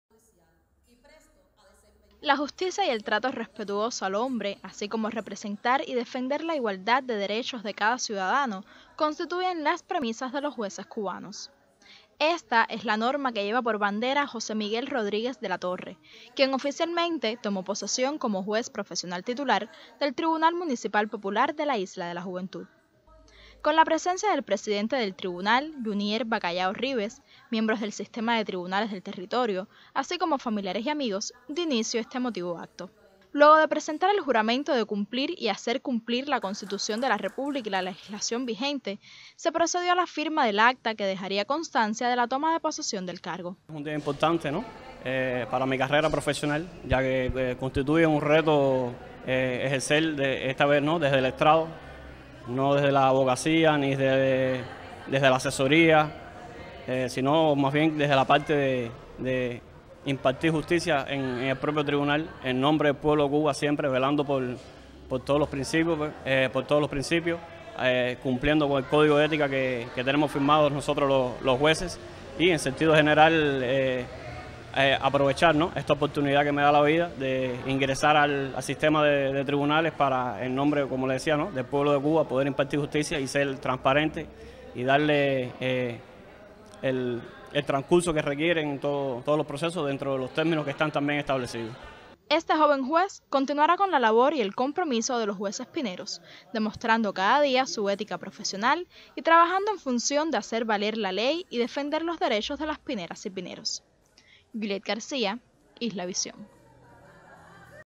Compromiso con la verdad en el cumplimiento de la ley
El compromiso con la verdad, el cumplimiento de la ley y el velar por los derechos de cada pinero, forman parte del compromiso que asume cada juez del tribunal municipal al tomar posesión de su cargo…